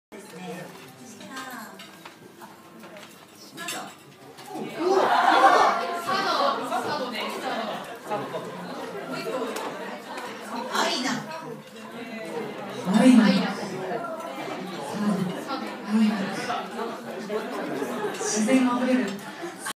エクセレントパーティー 一番小さなプレゼントのゆくえ NBC新潟ビジネス専門学校 広告ビジネス学科
NBC学生年間表彰会でのアトラクション景品。一番ちっちゃな物がありました。
どんな人がもらっていくのだろう？？